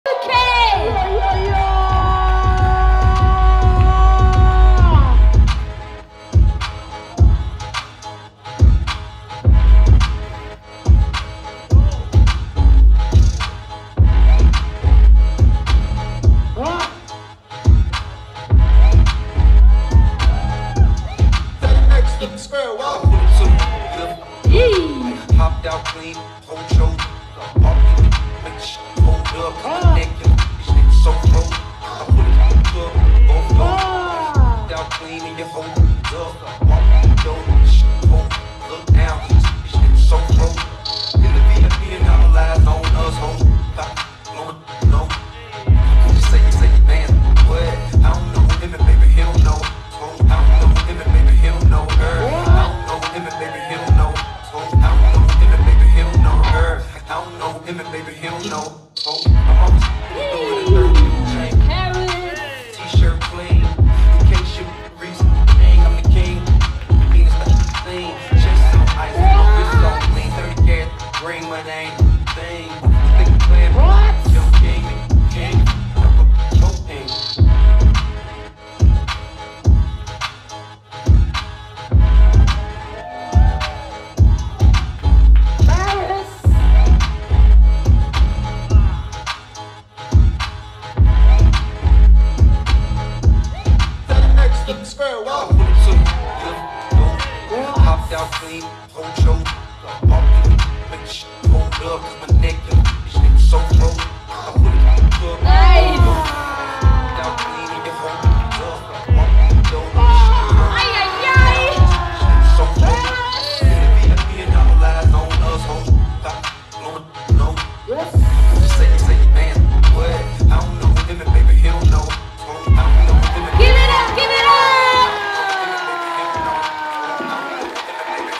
Paris | JUDGESHOWCASE POPPING | IMPROVE YOURSELF BATTLE 2024
JUDGESHOWCASE POPPING: Paris (UK)

This Event "Improve yourself Dance Festival" is organized by Stadtwerke Bochum in coorperation with Cypher44.
This  festival goes on for three days (from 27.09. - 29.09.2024) and is filled with Jams, Q&A Night, Battle & Workshops.

People from all over the world take part in this festival.
International dancers (HipHop & Popping) will compete on a dance floor under the critical eyes of an international jury.

The battle system is a simple knockout system.

JUDGES: Paris (UK) | Robozee (Germany) | Kyoka (Japan) | Evion (UK)
DJ: Bougito (France) | Ake (Italy)
HOST: Gifty Lartey (Germany) | Iman Gele (Germany)

LOCATION:
Gleis 9
Am Kulturgleis 9
44787 Bochum

If you don't want to miss out on future festivals, events and projects, check out their Instagram for more information.

Also shout out to GangstaSoul for helping at the event.